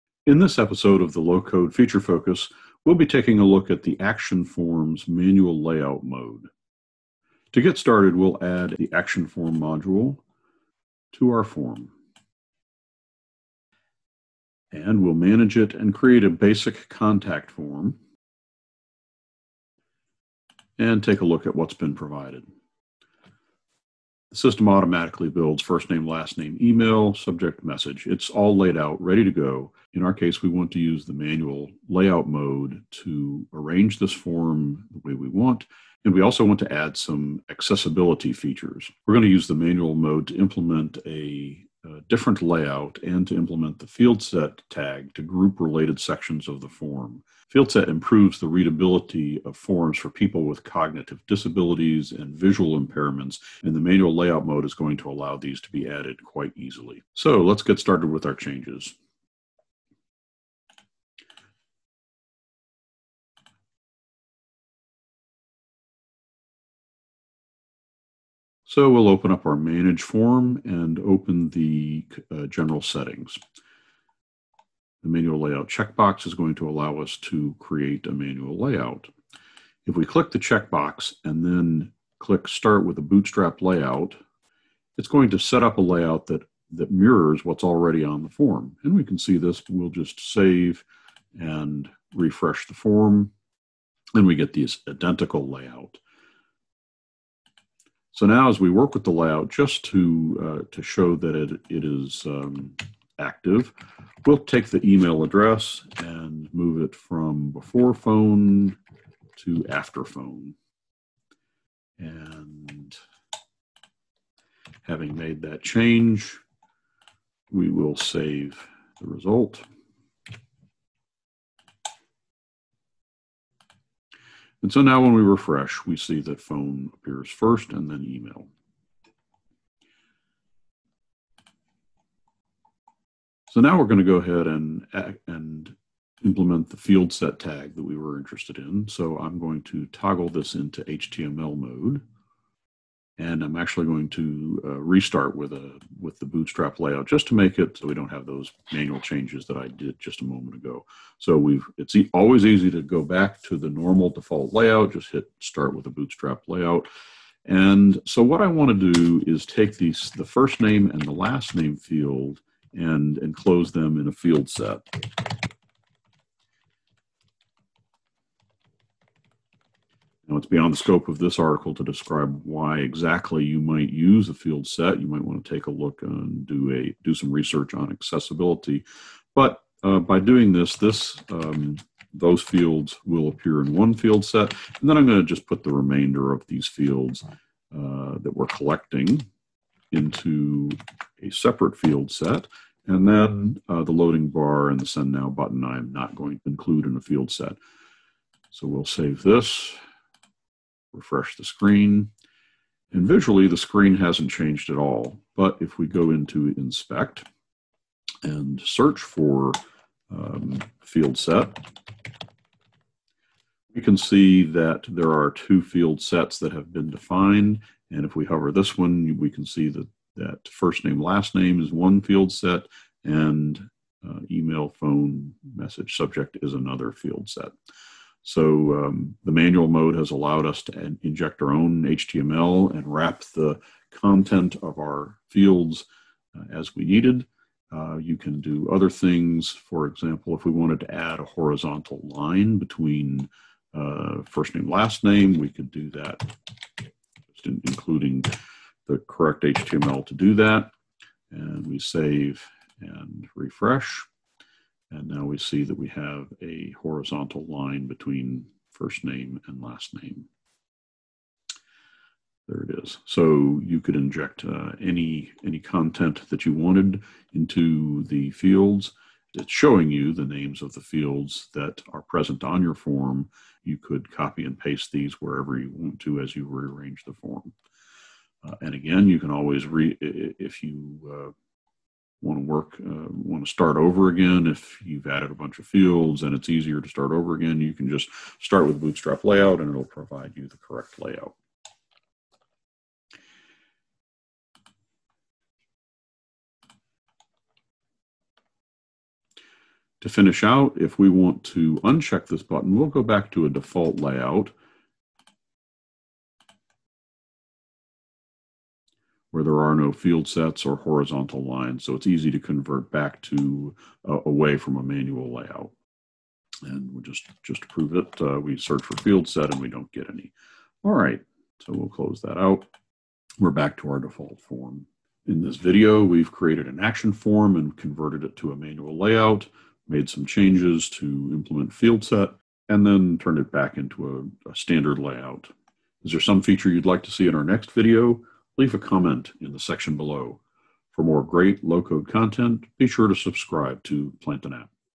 Low-Code Feature Focus: Action Form Manual Layout
In this video, our US Head of Support, Dale, creates an Action Form and converts it to a manual layout to implement the fieldset html tag to group related sections of a form. Fieldset improves readability for people with cognitive disabilities and visual impairments, and the manual layout mode allows these to be added quite easily.

00:00 Introduction
00:29 Describing the changes we want to make
01:00 Converting to a Manual Layout
02:13 Customizing the HTML with the fieldset tag
04:59 Converting back to an automatic layout
05:24 Summary